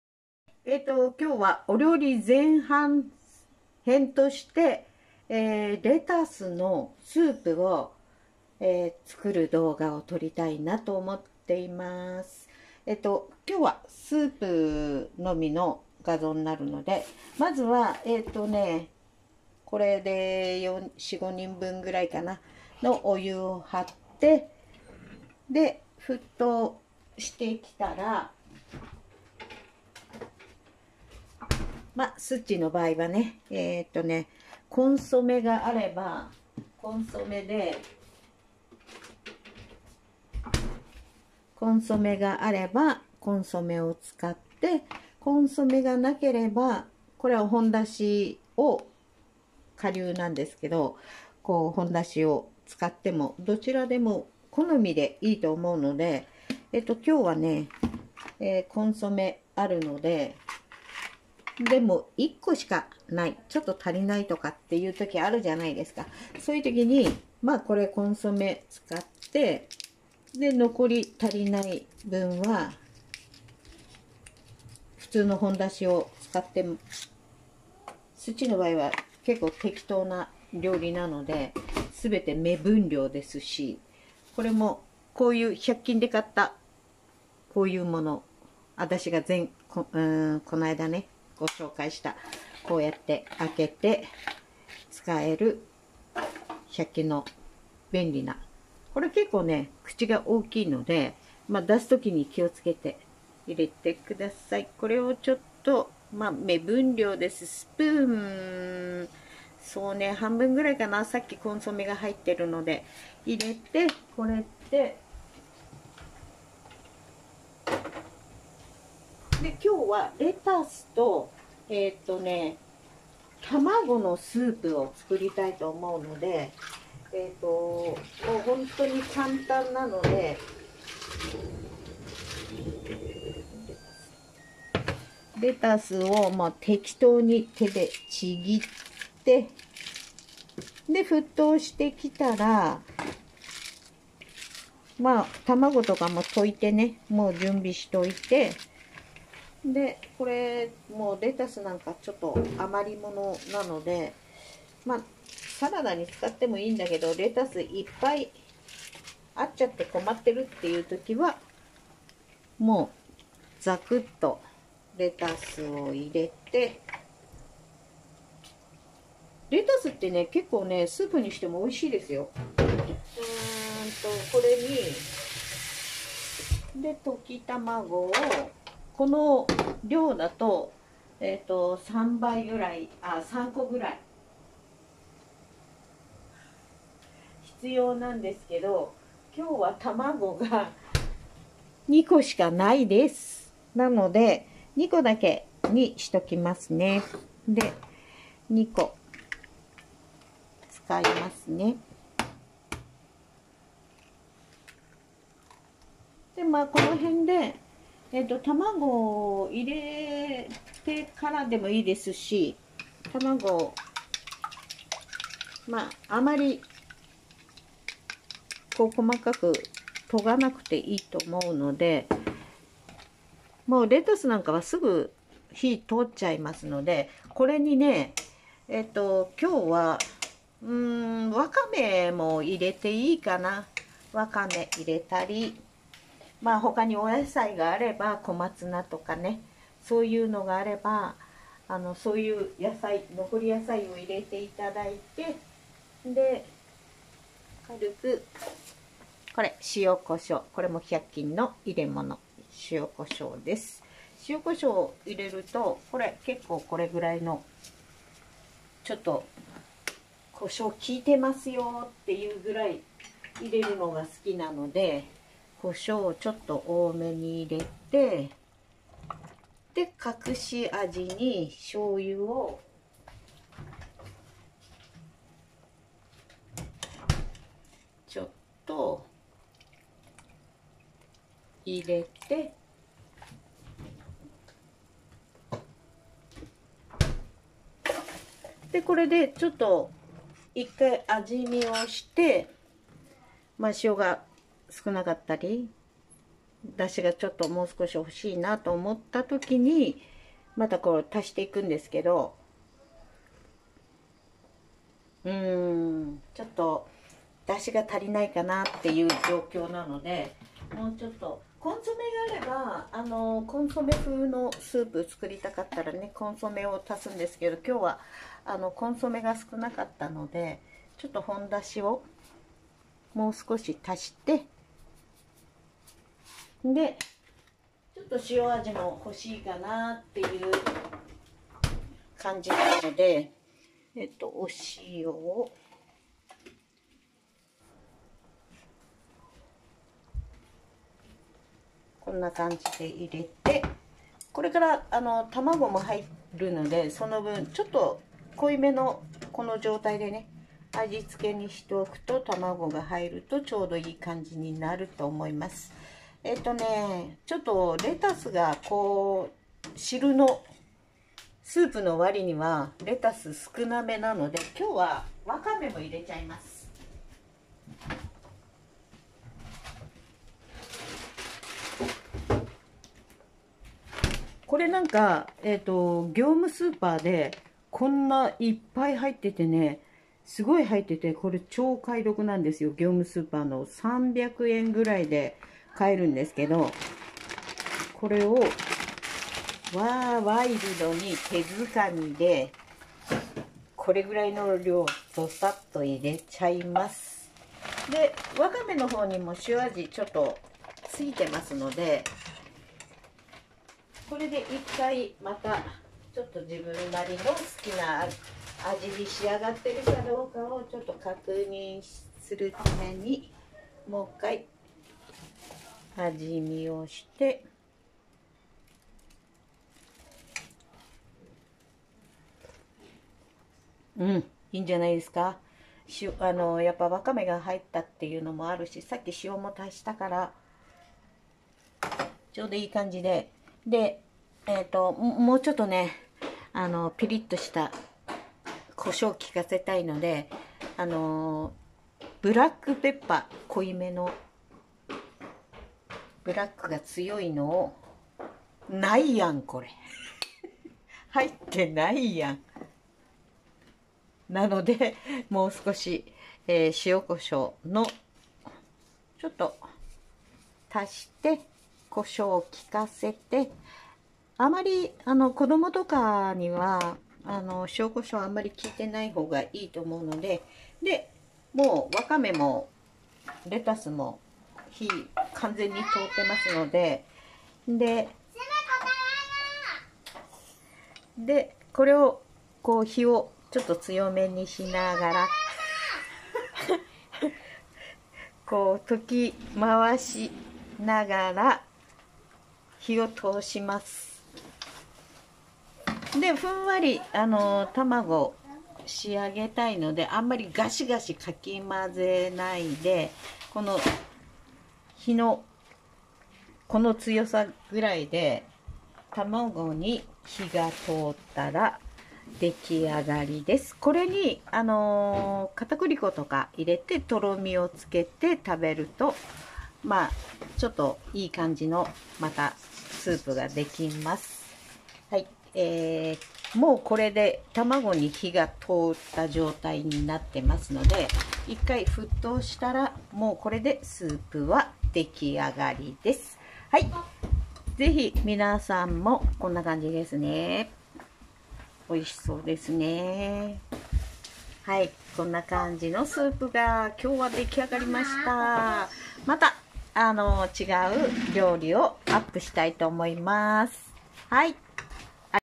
#[簡単料理#レタス、ワカメ、卵スープ料理に挑戦!
なんと!初企画!スッチーのお料理企画が初公開です!

少しでも参考にしてもらえると嬉しいです＼(^o^)／
後半はみんな大好き○○の料理を作ったので是非そっちも見てね♥♥